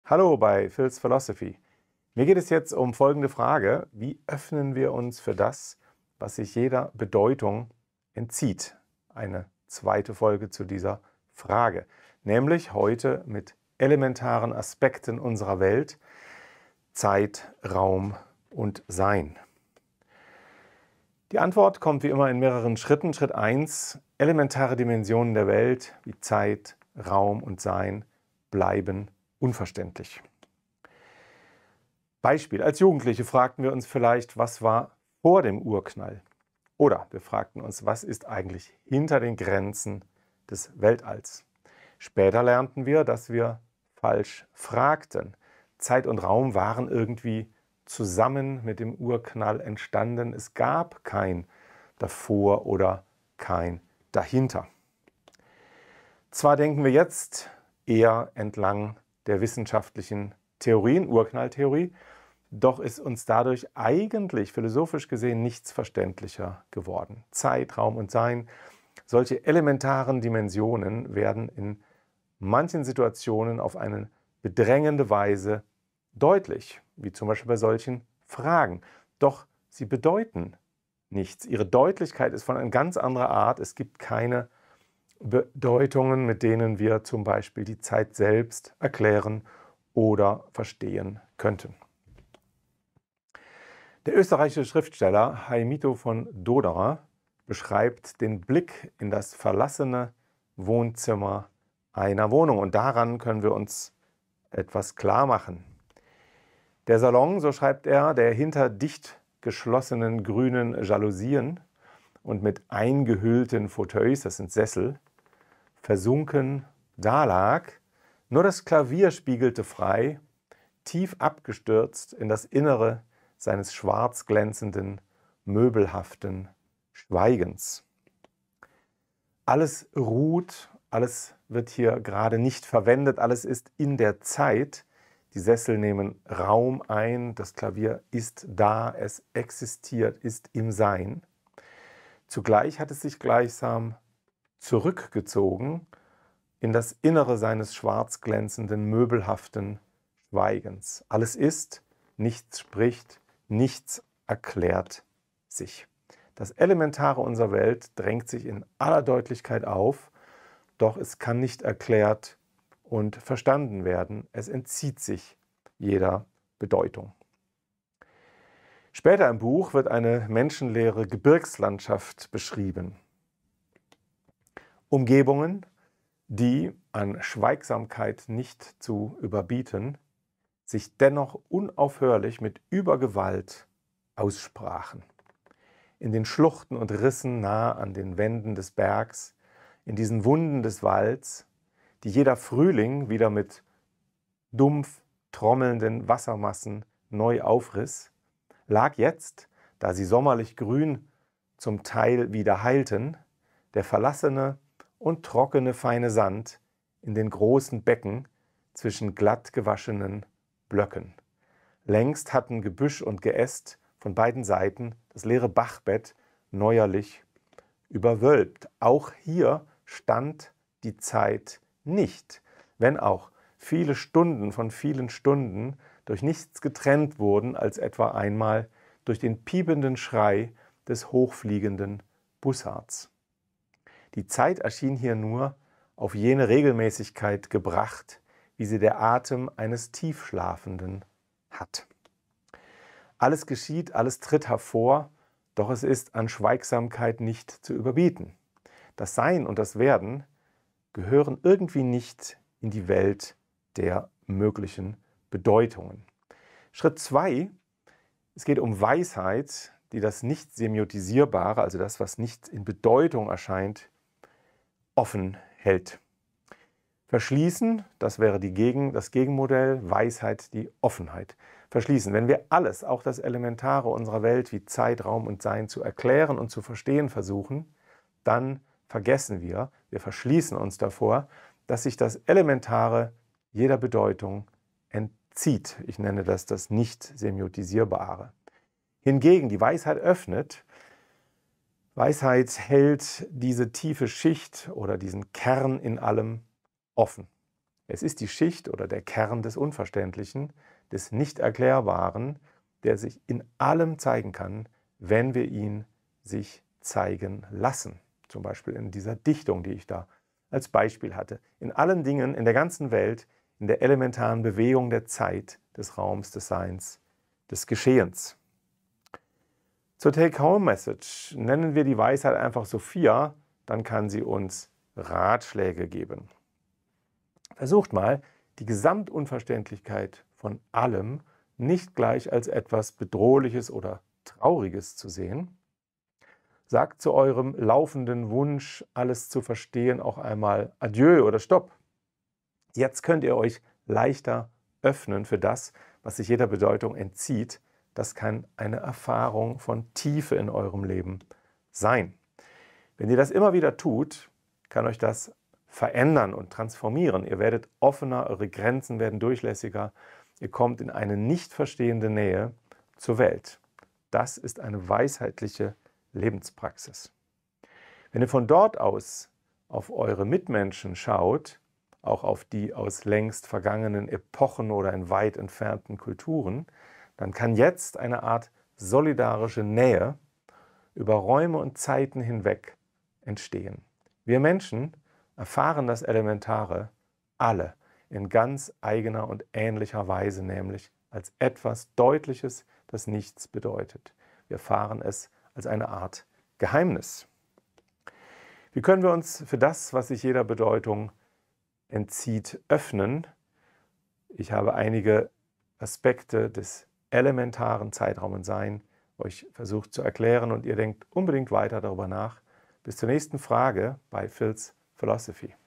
33 Wie öffnen wir uns für das, was sich jeder Bedeutung entzieht? (II) (Zeit, Raum, Sein)
Wie öffnen wir uns für das, was sich jeder Bedeutung entzieht? In der zweiten Folge geht es um sehr allgemeine Begriffe oder Aspekte unserer Wirklichkeit, um die Zeit, den Raum oder das Sein. Zwar gehören diese Aspekte ganz selbstverständlich zu unserer alltäglichen Welt. Doch sie sind keine Dinge in dieser Welt, sie haben auch keine Bedeutung in dieser Welt, so wie die Dinge, sondern sie liegen der Welt auf schwer verständliche Weise zugrunde. Wenn wir darauf verzichten, diese unverständlichen Aspekte mit Macht erklären zu wollen, etwa indem wir sie mit der Begrifflichkeit bestimmter Weltbilder (z.B. religiöser) kolonisieren, dann können wir uns ihnen auf ganz andere und neue Weise öffnen. Hier geht es um Praktiken der Weisheit.